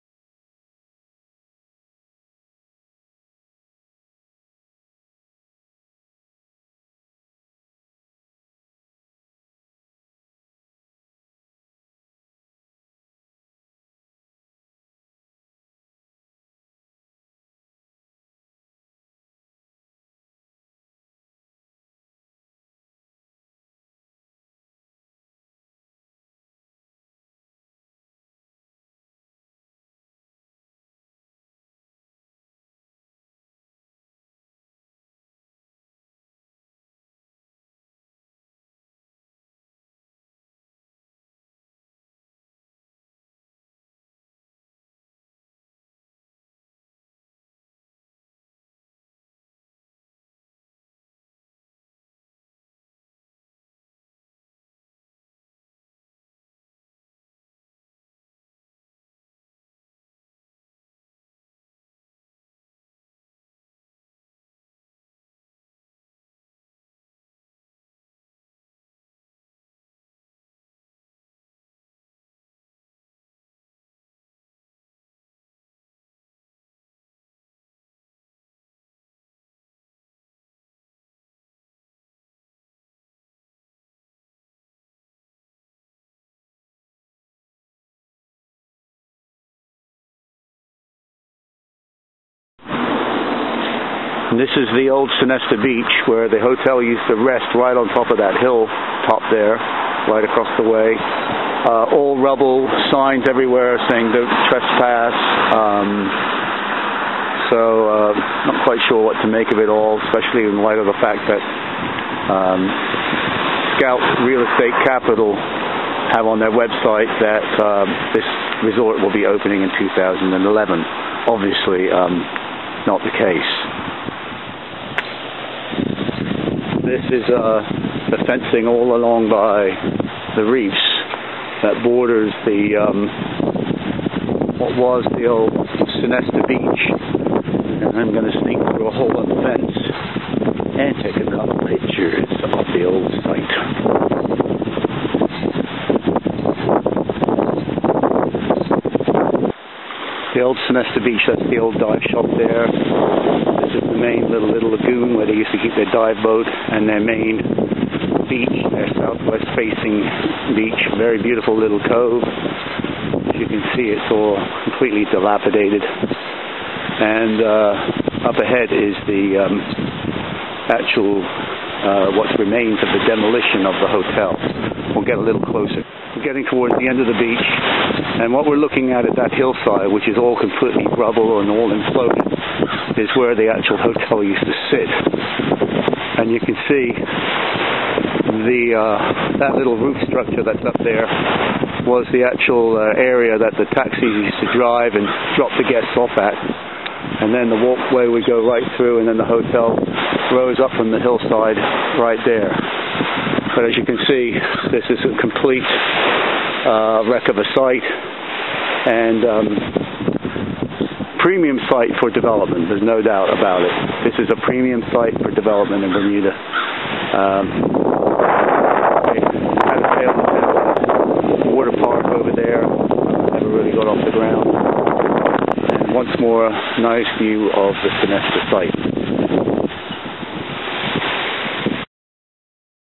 Sonesta Site - perfect for tourism development in BERMUDA
When Lehman Brothers collapsed so did the Southampton Beach Resort. There is a website Scout real estate caiptal that claims the resort is oppening in 2011.
This is obviously incorrect.
Whatever the status, there is NO DOUBT that this site is absolutely awesome and a fabulous resort powered by a STAR developer, consortium/brand is certainly what should be happening here.